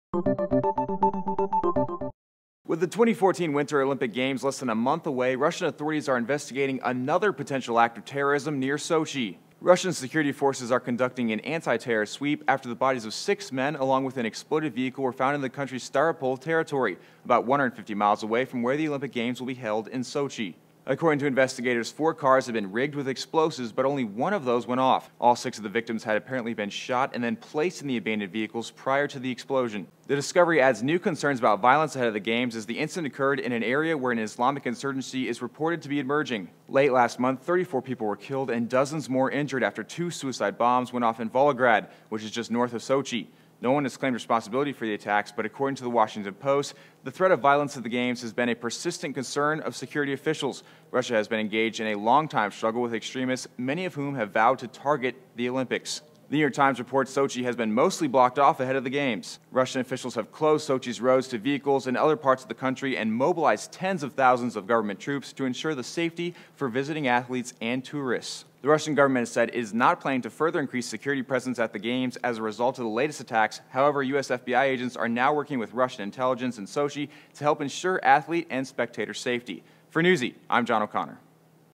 Russia Launches Probe After Six Found Dead Near Sochi
With the 2014 Winter Olympics less than a month away, Russian authorities are investigating another potential act of terrorism near Sochi.